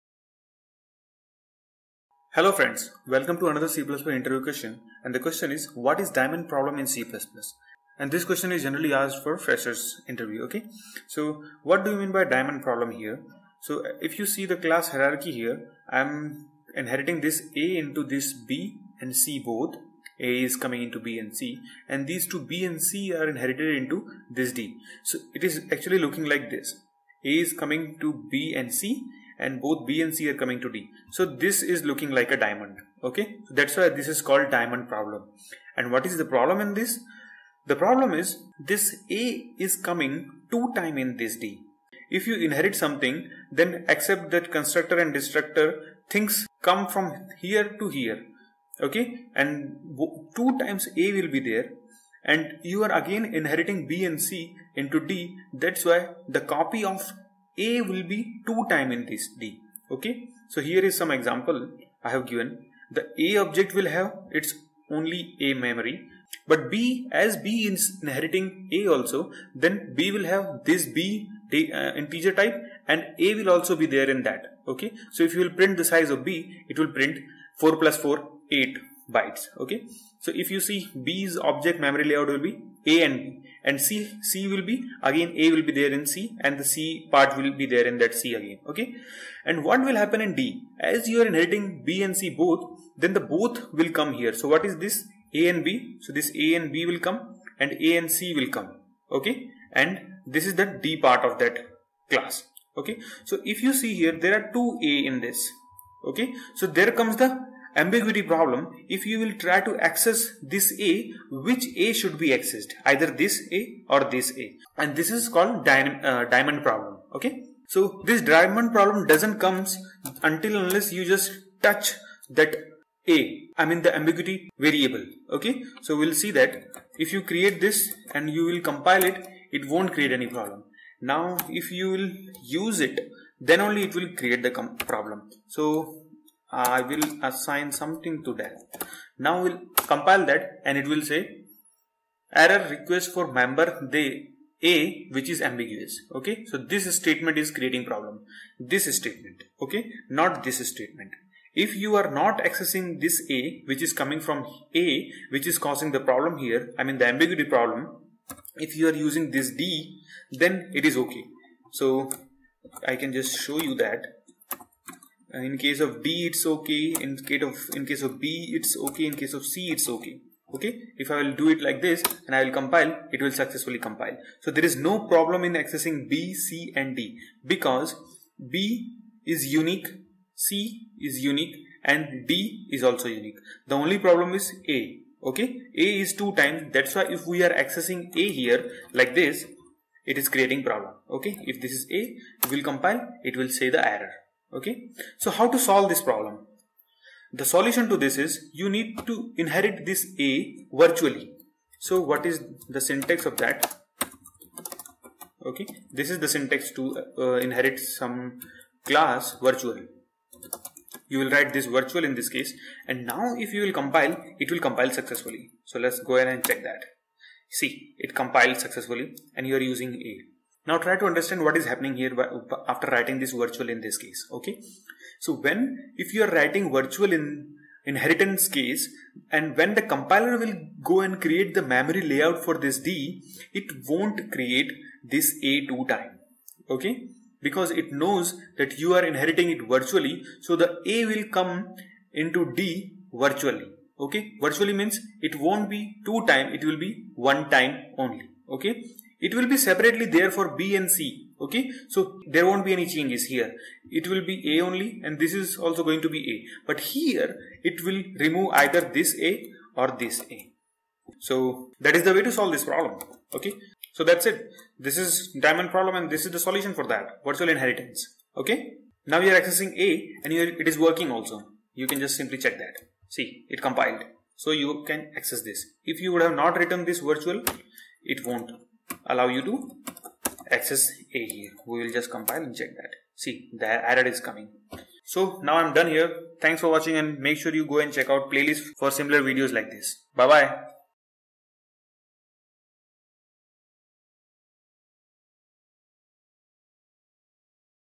Diamond Problem In C++
JOIN ME

In this video we will see very famous problem call diamond problem in C++ programming language. This is related to inheritance when we have common parent class and two child class are inherited into one class we face this problem.